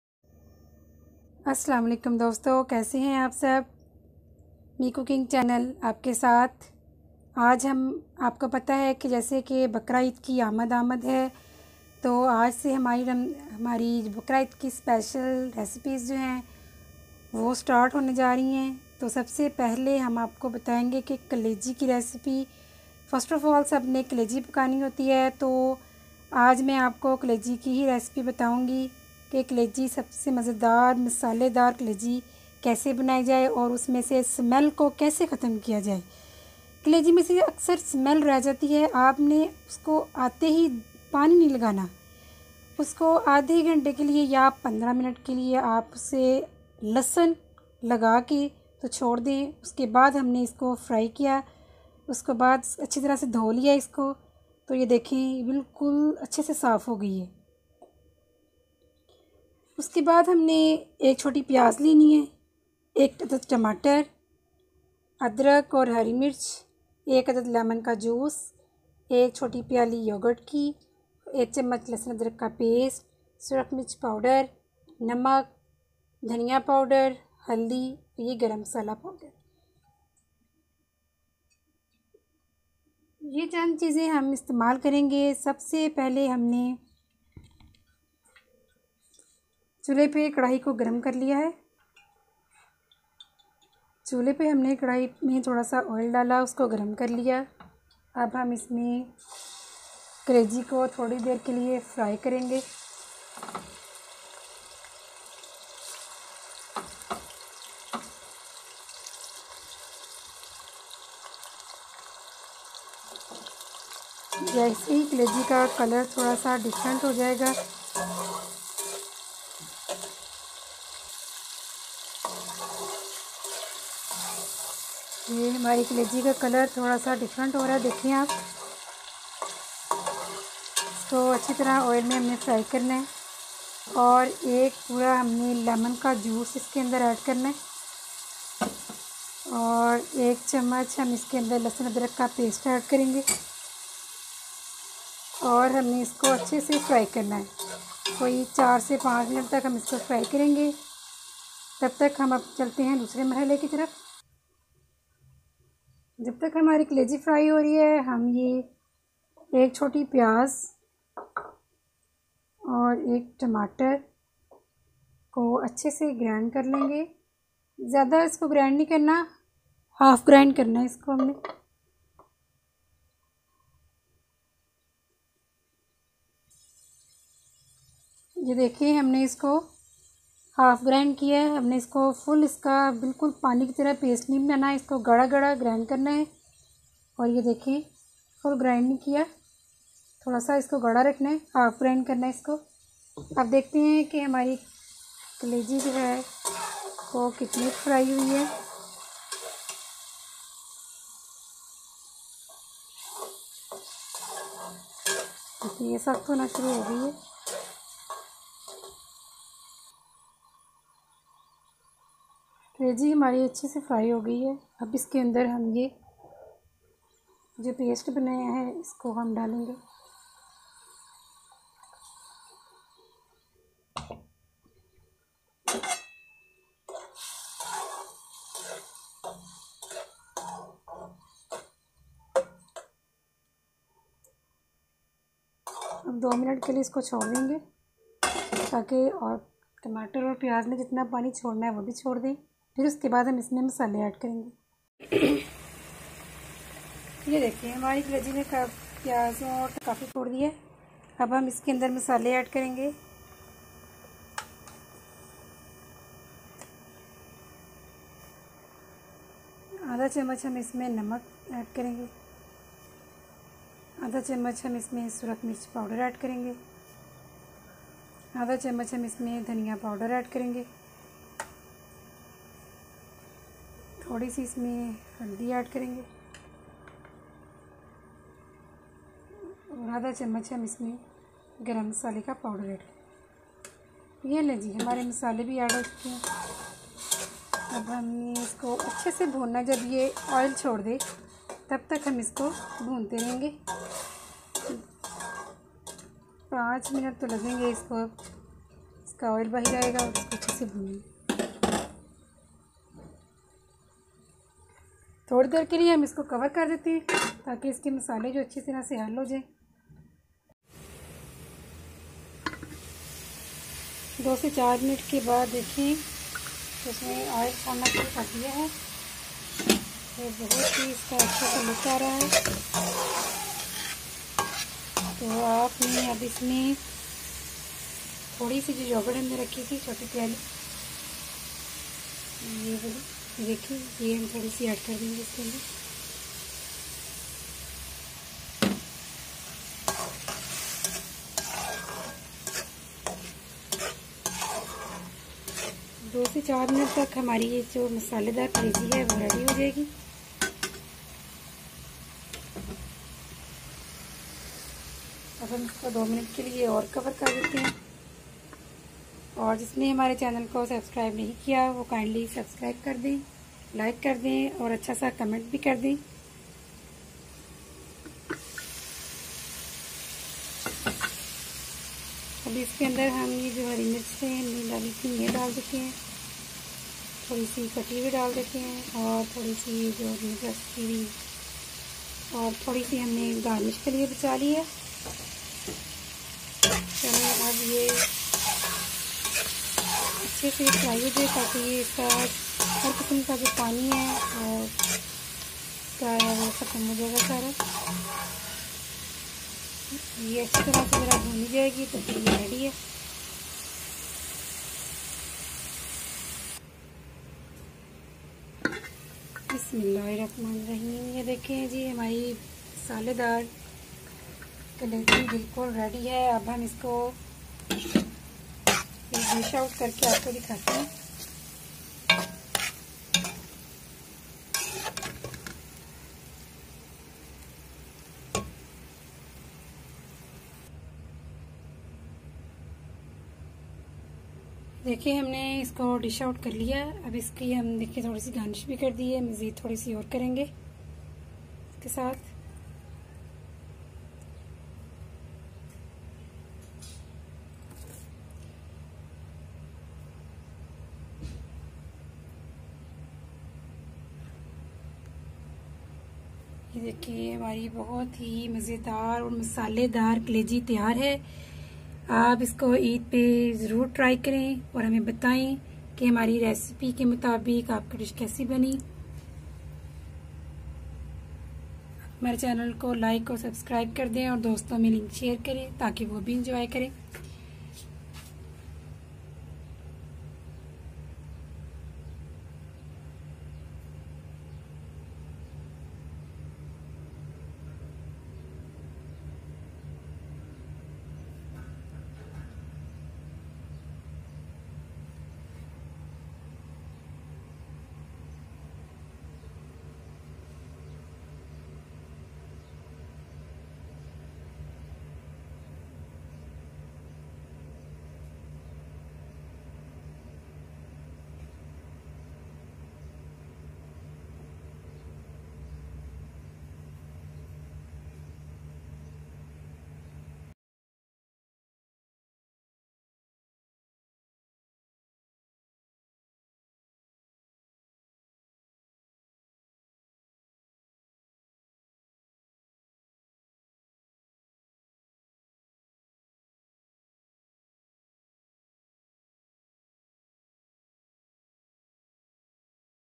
Kaleji Masala Recipe - Mutton Kaleji bananey Ka Tarika - How To Make Mutton Kaleji On Eid Special
Mutton Kaleji by Me Cooking and
How to make kaleji recipe at home very easily. Quick and easy. Follow all step in video. It is beneficial for learners.
Ingredients mention in video.

I Hope Everyone is Fine By The Grace Of Allah Almight